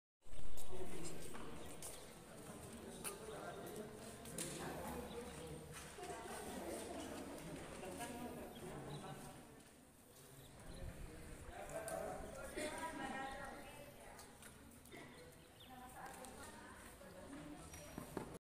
Ciudadanos emiten su voto en casillas del municipio de San José Miahuatlán
Ciudadanos emiten su voto en casillas del municipio de San José Miahuatlán en donde cuatro candidatos buscan la presidencia municipal.